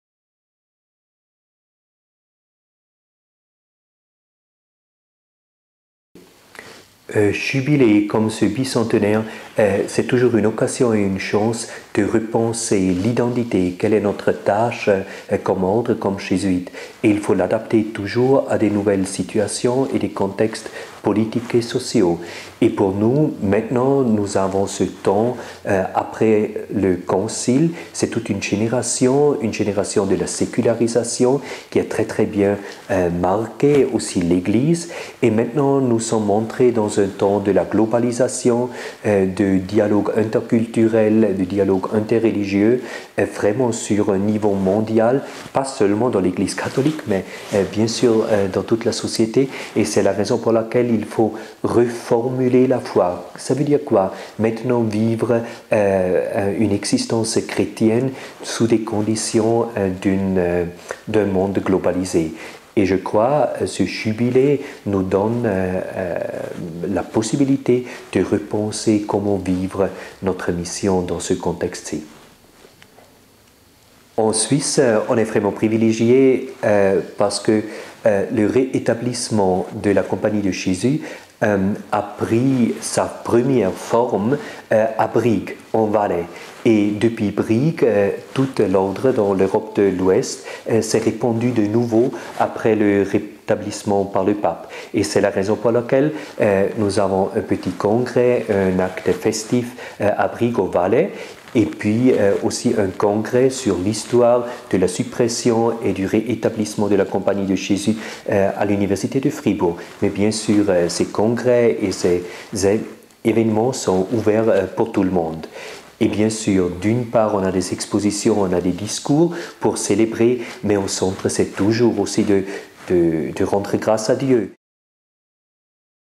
Entre réflexion et célébration
2014, le temps de la commémoration de la restauration de la Compagnie de Jésus. Quels sens  donner à ce jubilé ? avec Christian Rutishauser sj, provincial de Suisse.